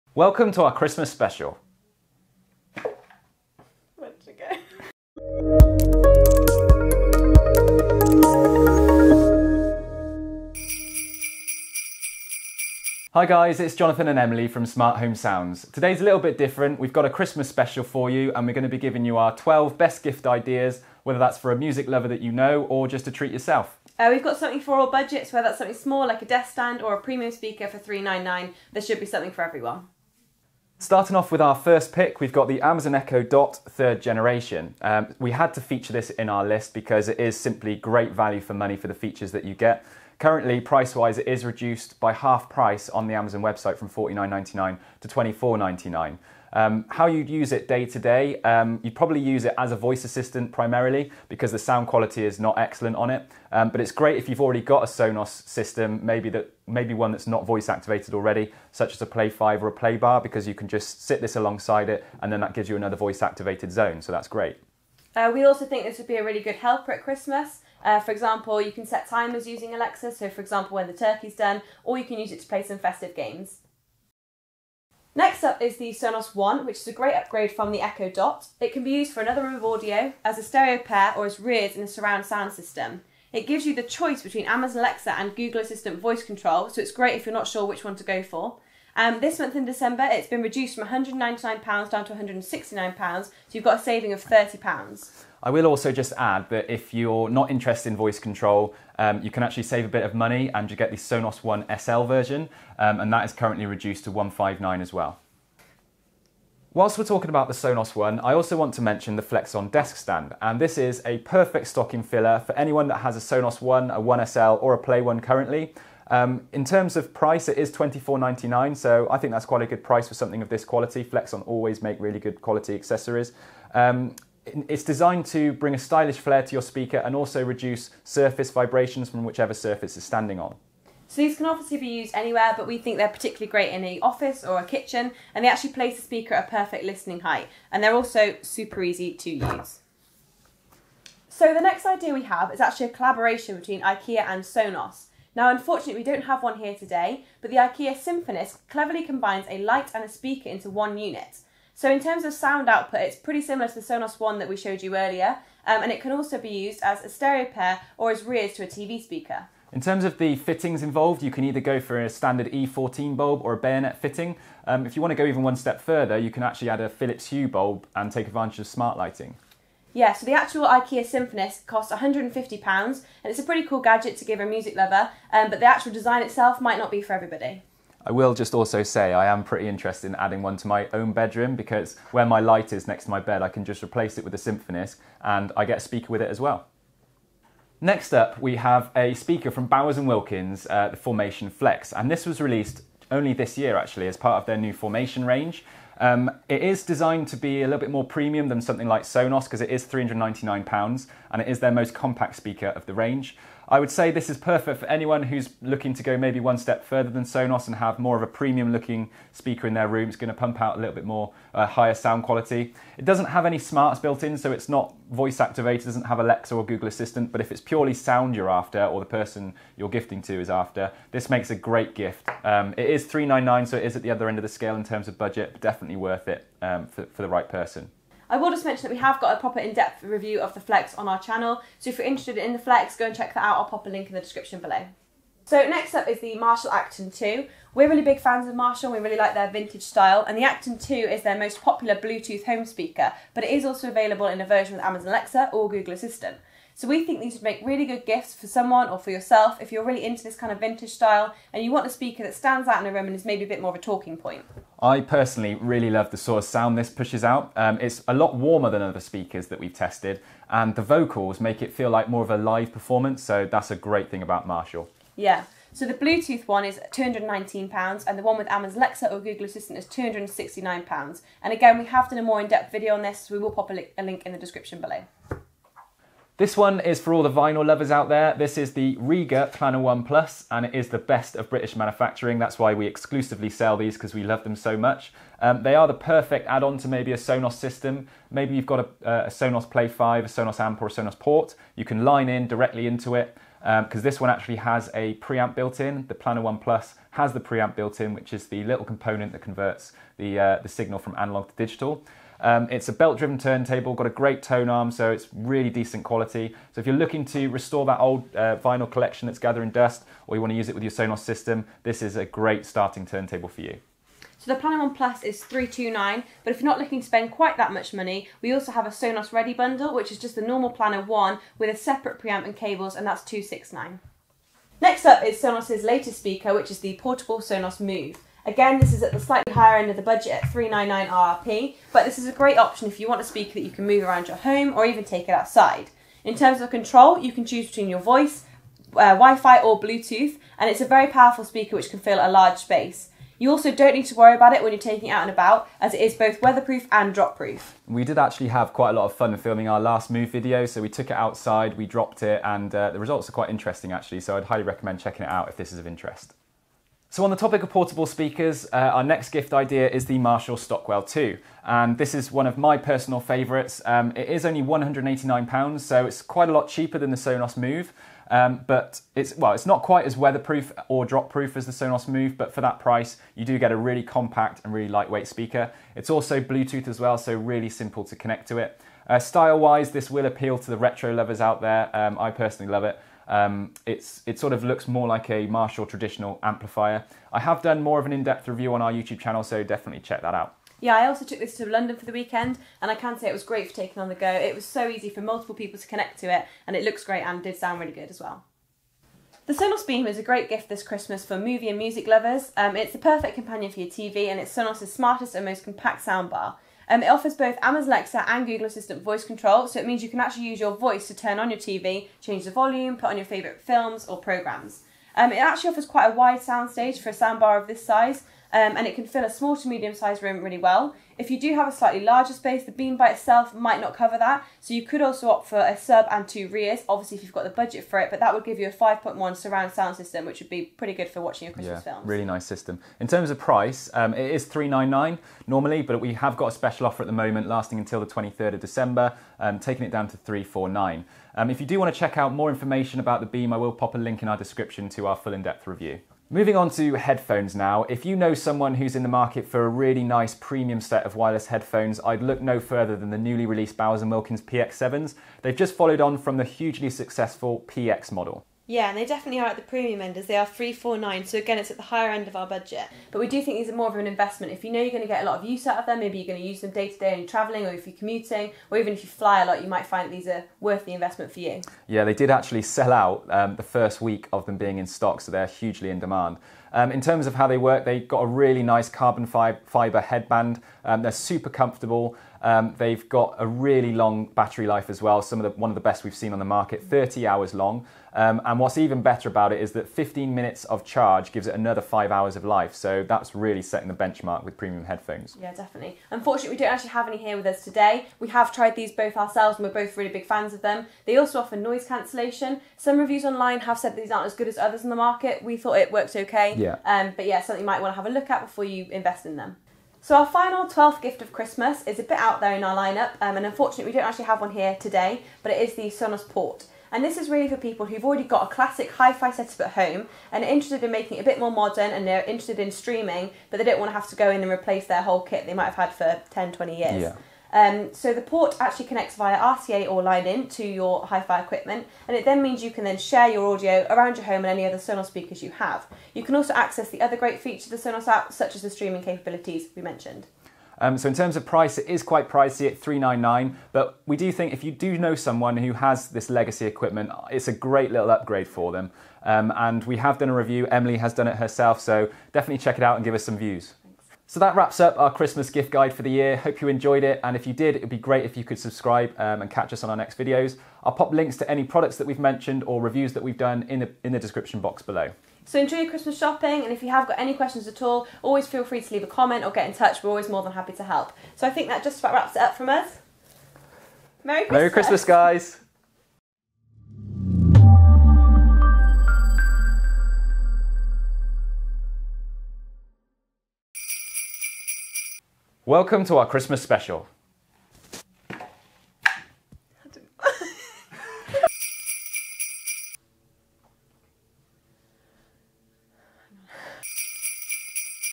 Top 12 Christmas Gifts for Music-Lovers | Feat. Sonos, Marshall, Amazon & more!
Our top 12 picks that we think would make great Christmas gifts for 2019!

1. Amazon Echo Dot

3. Flexson Desk Stand for Sonos One

4. IKEA Symfonisk Lamp

5. Bowers & Wilkins Formation Flex

6. Marshall Acton II Bluetooth

7. Rega Planar 1 Plus turntable

11. Bowers & Wilkins PX7 Headphones